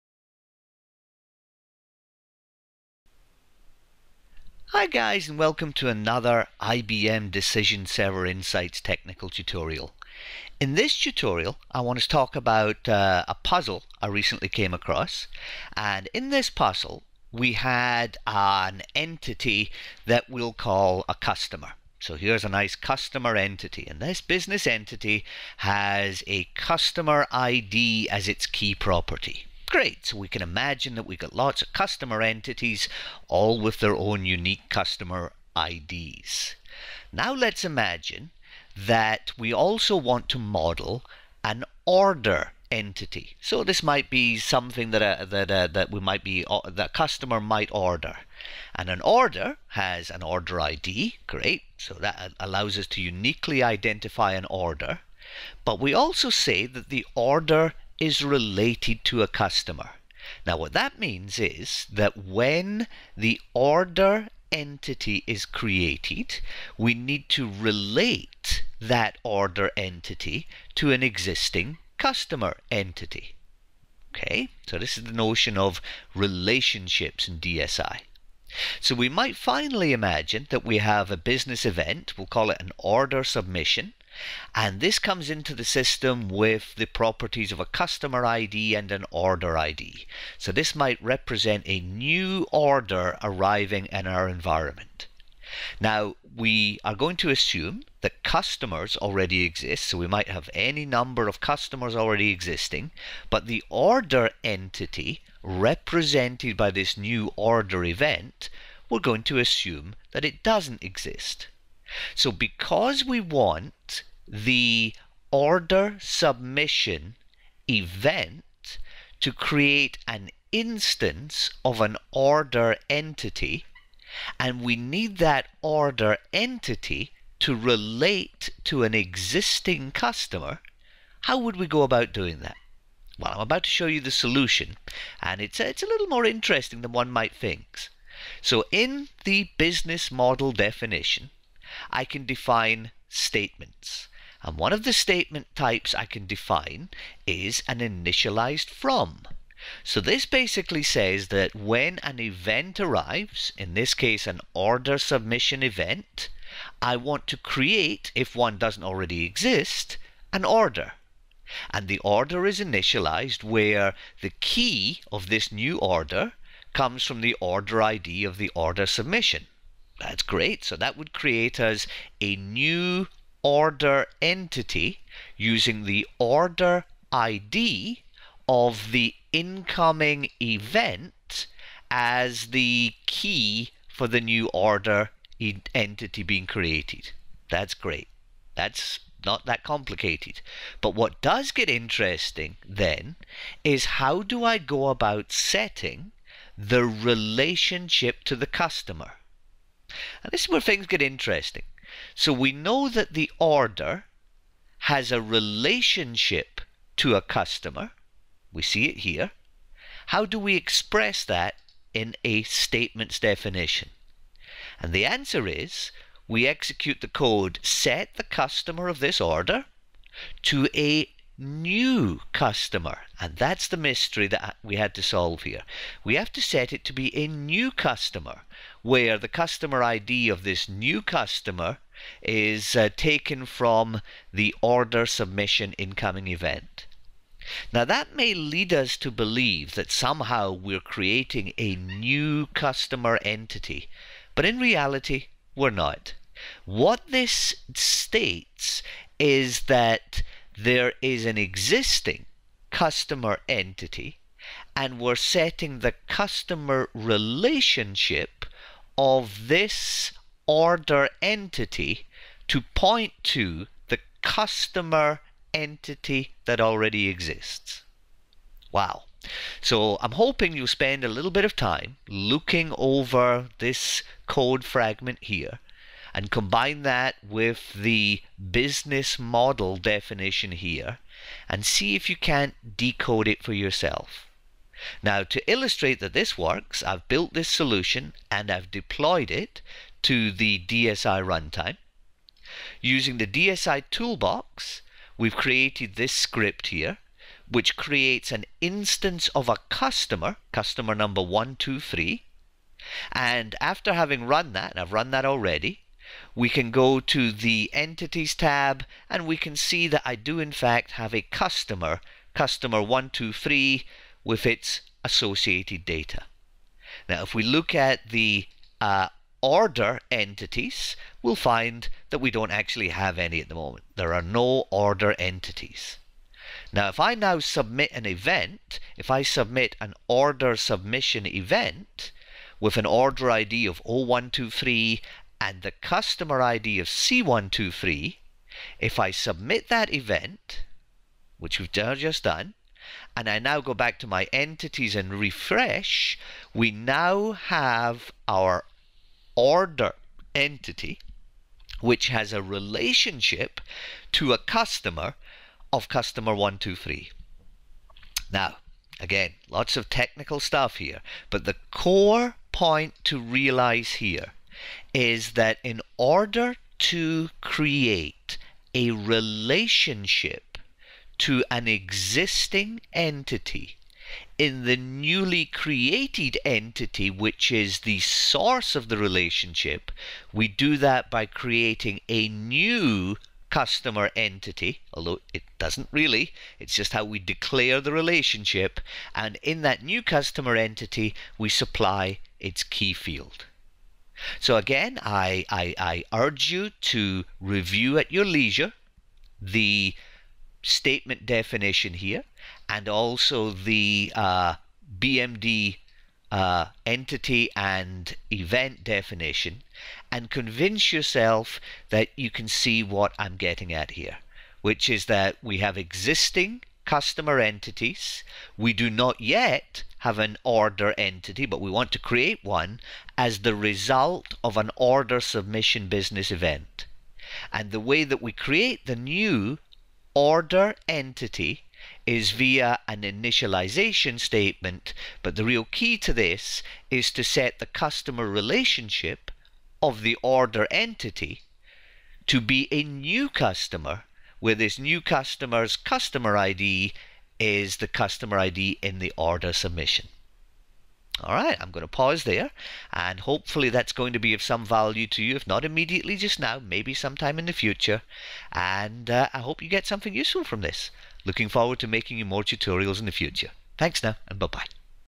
IBM DSI: Related Entities
Technical tutorial on the IBM Decision Server Insights functions.  This tutorial describes how to create relationships to an existing entity when creating a new entity as the result of an arriving event.